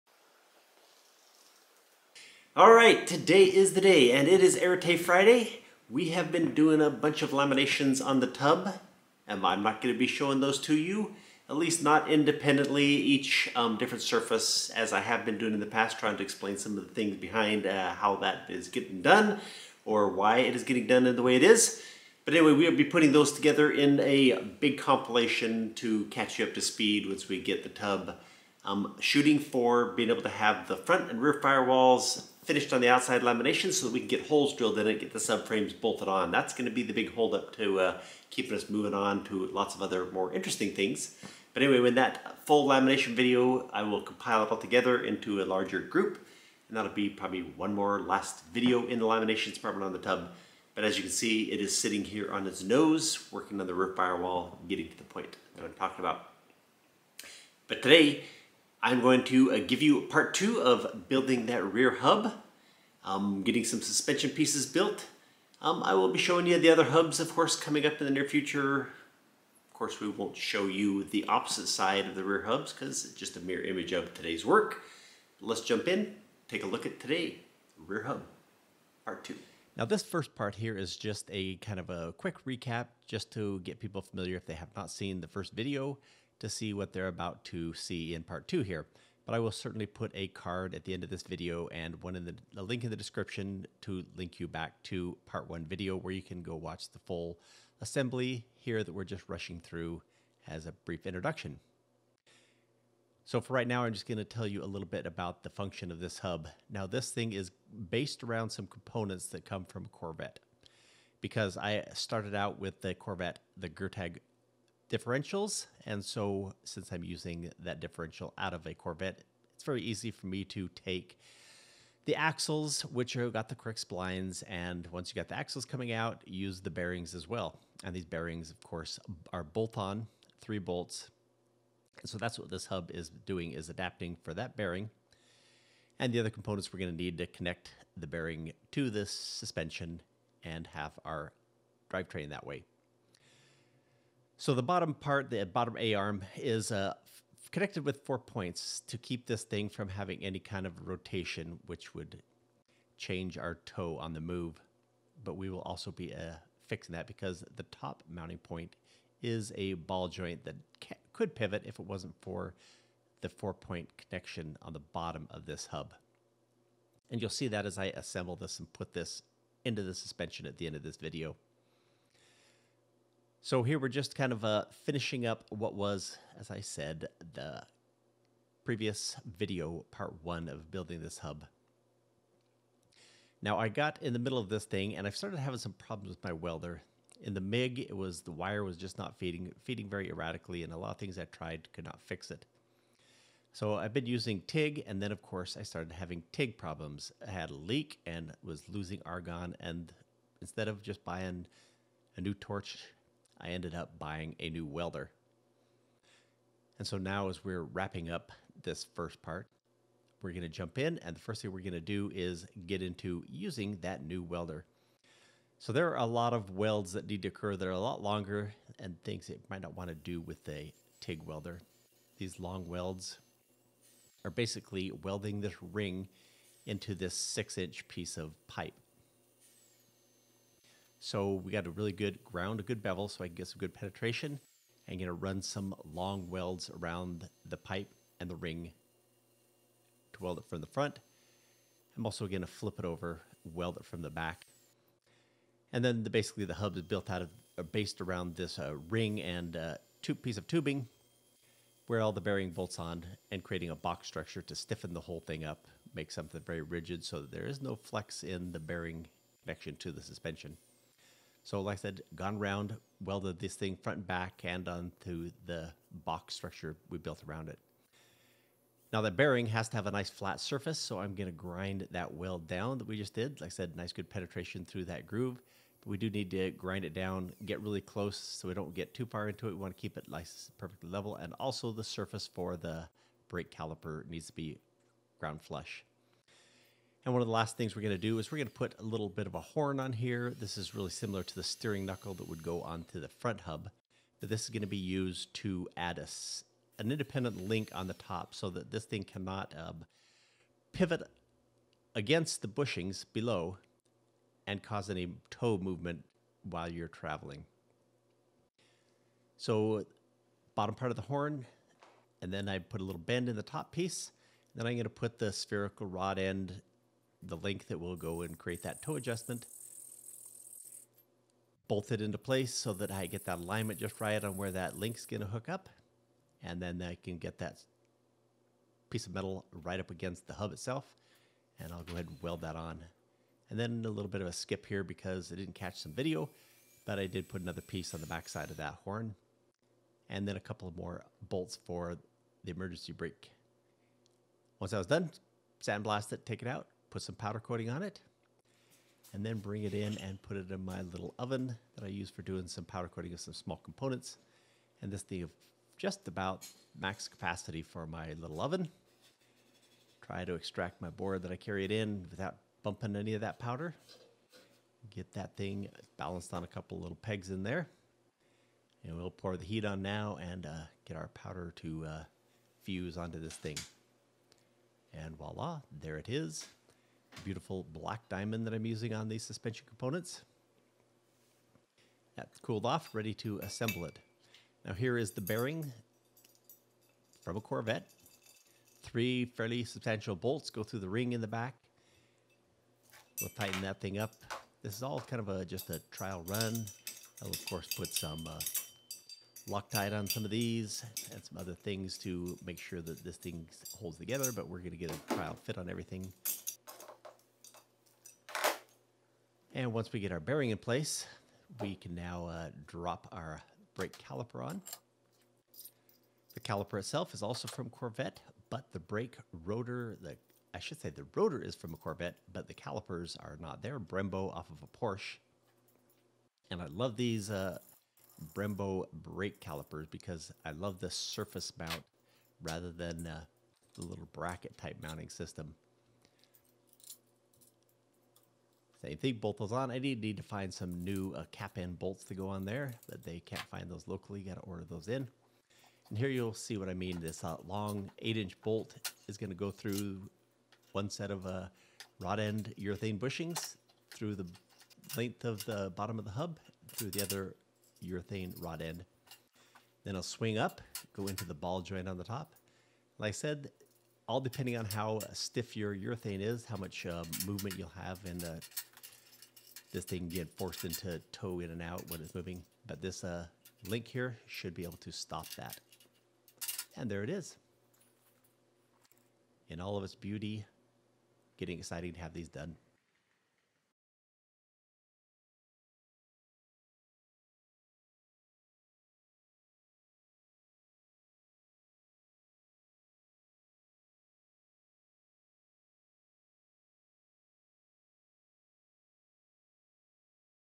A77 Lamborghini parts off Ebay? Na! Build your own rear hub- part 2. Arete Supercar project
I build my own hubs for less than $100. Thinking about buying parts from some other car off Ebay or the wrecking yard? Beware! The all wheel drive Lamborghini Huracan parts just might have worked, but then I would have had to use the Lamborghini control arms and then alter my pick up points and then . . and then . . a never ending spending cycle. In this video; with a little more thrift in aftermarket parts (think bearings, rotors, etc.) and some good ole manual labor, we build our first wheel hub. This video is part 2 of that fabrication.